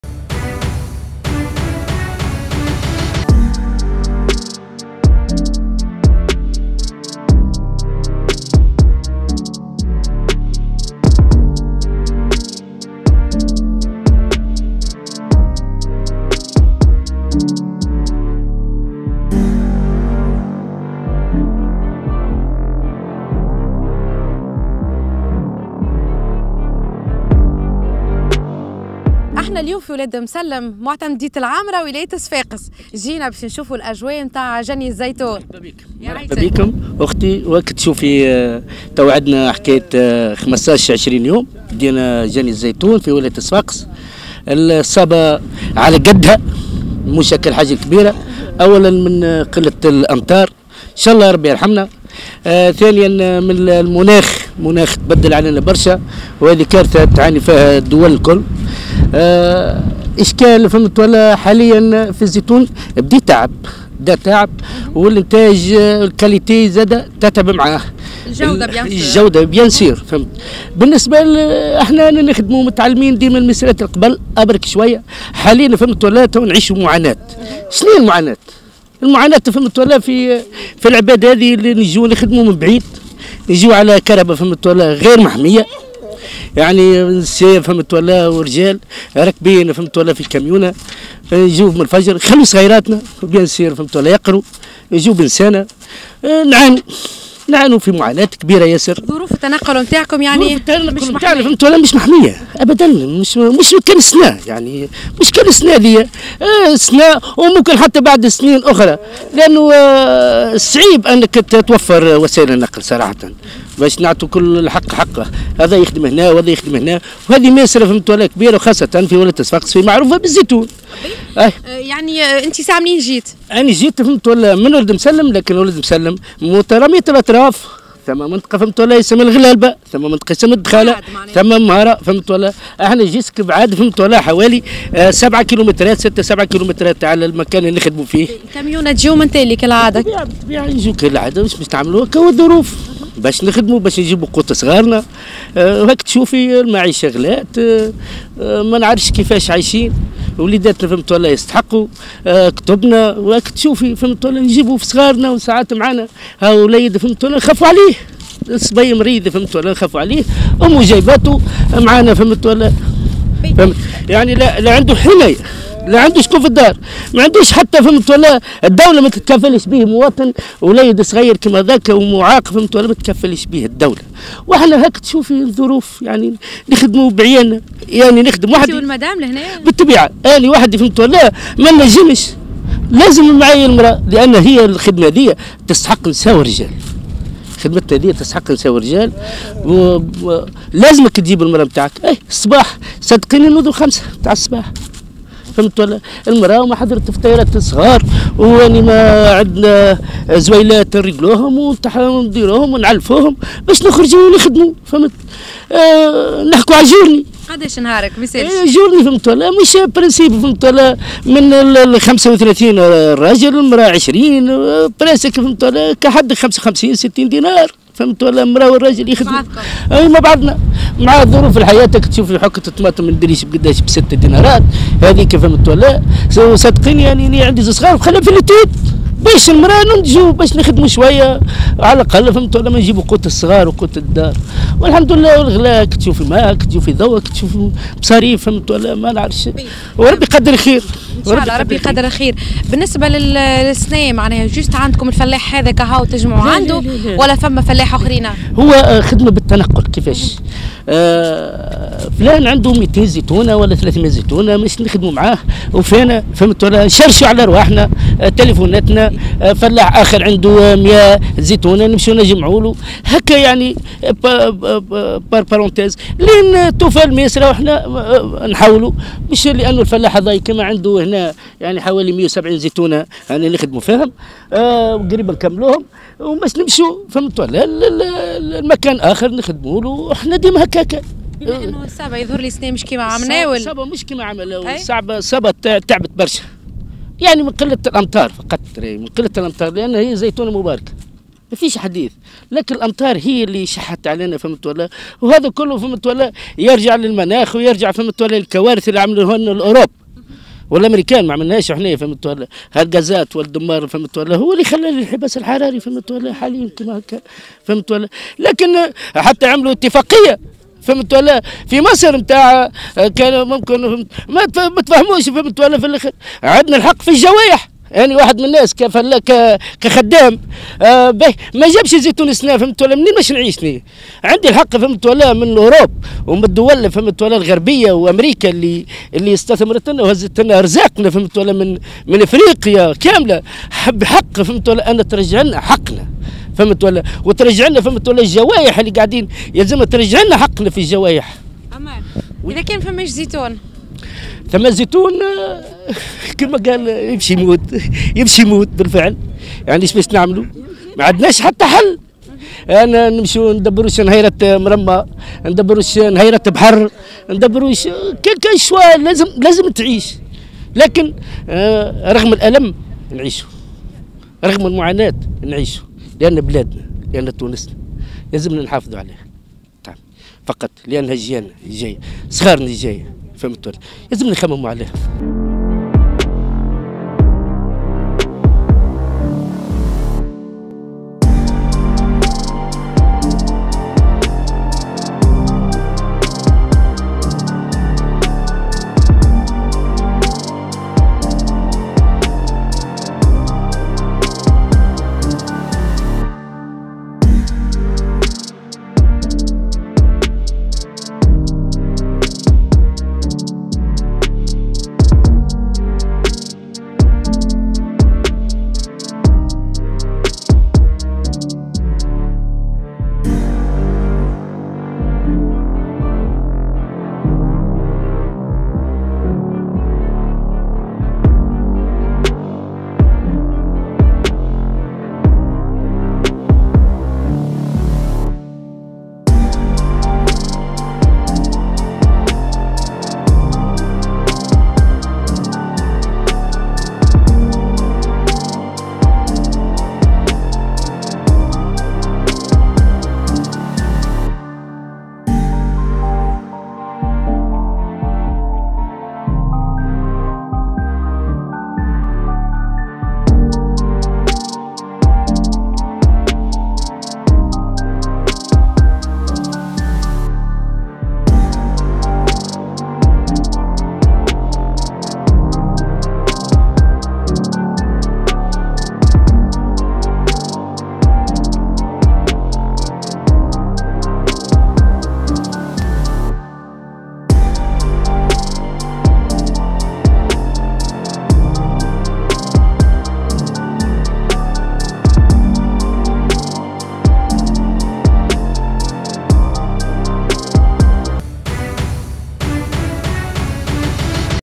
جولة على االمباشر في غابات صفاقس..ظروف تنقل قاسية لجني الزيتون وشح المياه أثر على الصابة هذا العام!!
جولة على االمباشر في غابات صفاقس
ظروف تنقل قاسية لجني الزيتون وشح المياه أثر على الصابة هذا العام